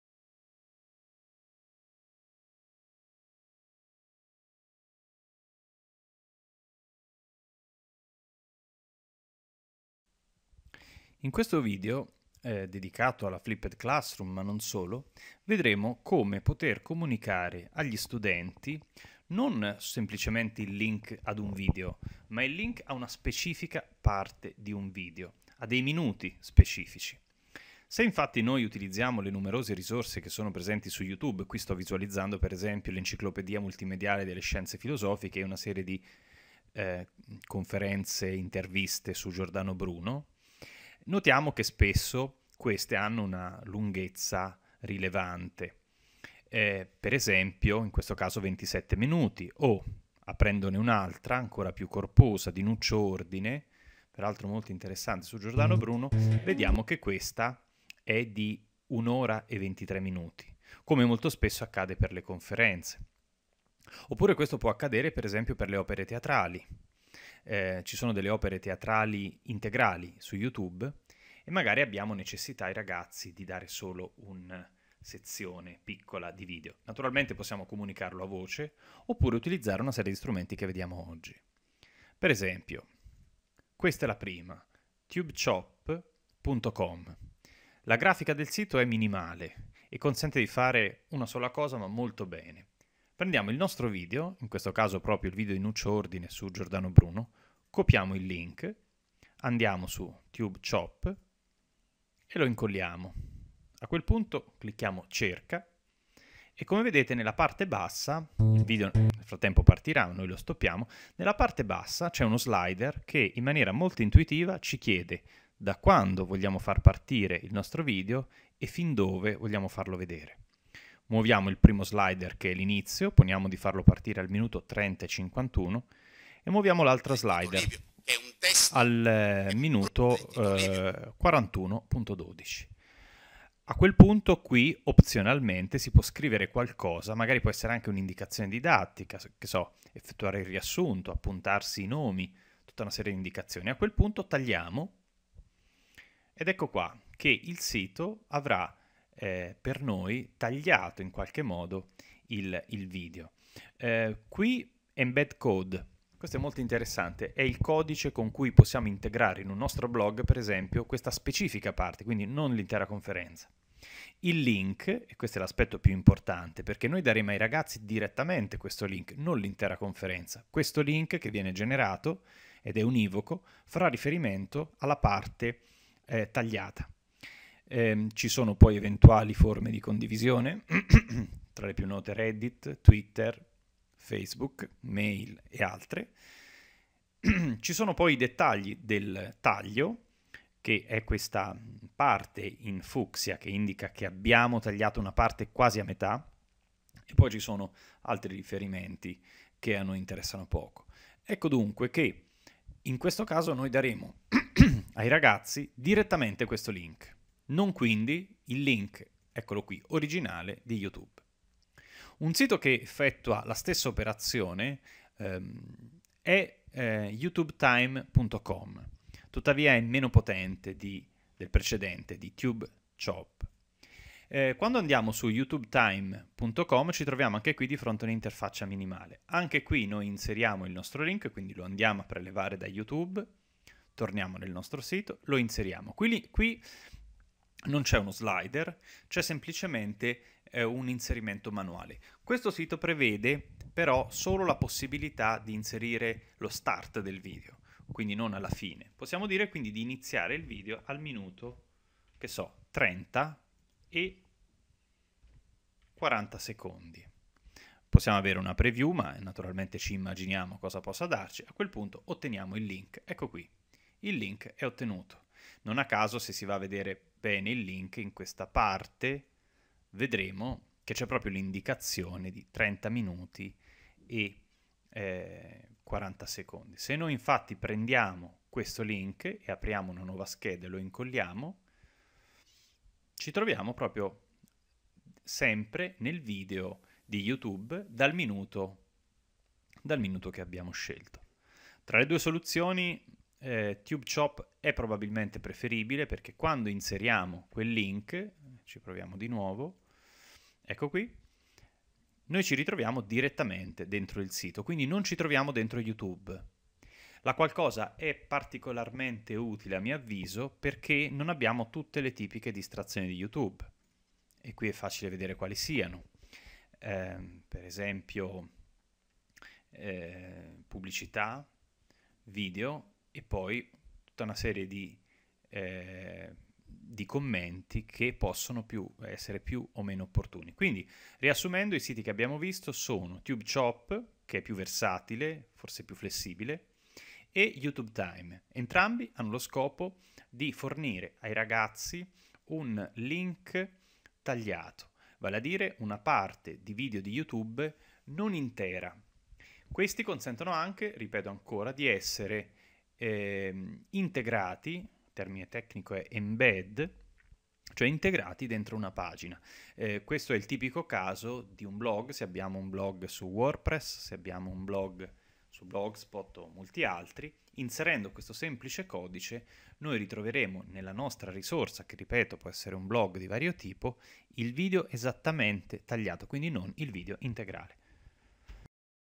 FLIPPED CLASSROOM tutorial: visualizzare solo una sezione di video di youtube (By DesignDidattico)
Il video illustra come dare agli studenti solo frazioni di video di youtube, utile per concentrare l'attenzione su specifici minuti